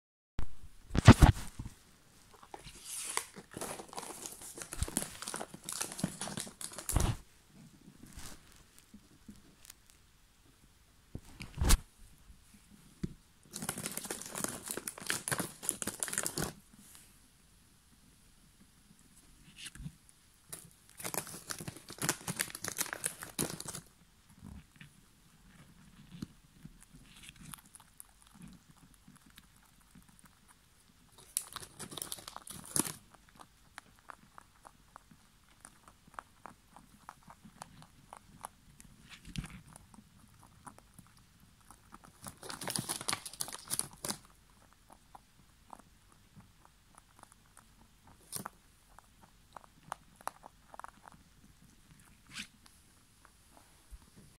Fluffy bunny wants food (and gets it...)
This is my fluffy bunny Luna wanted her treats for the day :D